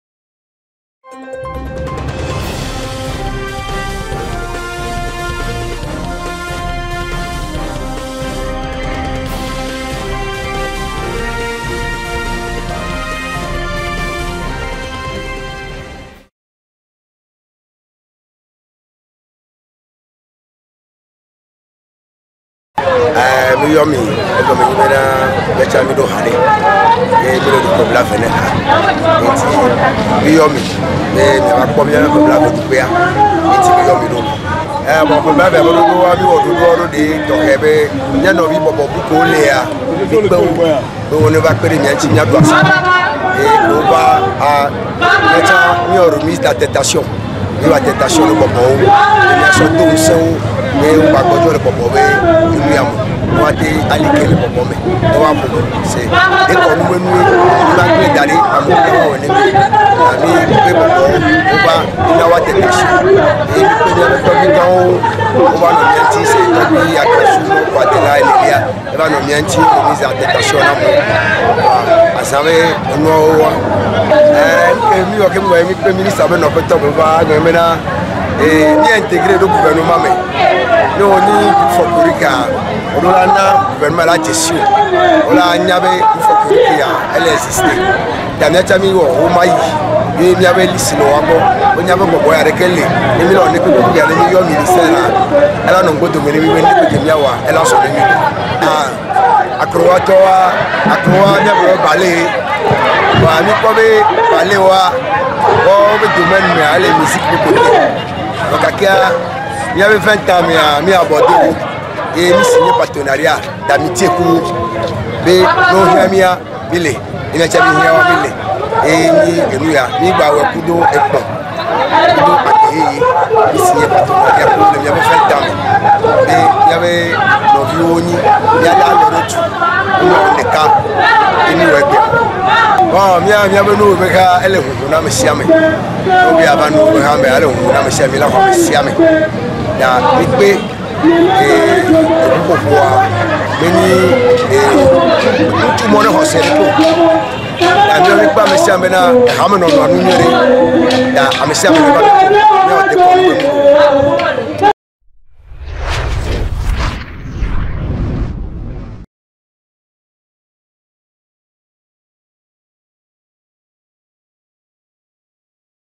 TOGO-CULTURE / ADJOGUIDJI Komlanvi Djito donne ses impressions à la célébration
Le groupe folklorique ATSOGBADJA de Bè N'Tifafa Komé a feté ses 20ans d'existance du 23 au 24 Septembre 2023 dans le quartier Bè N'Tifafa Komé à Lomé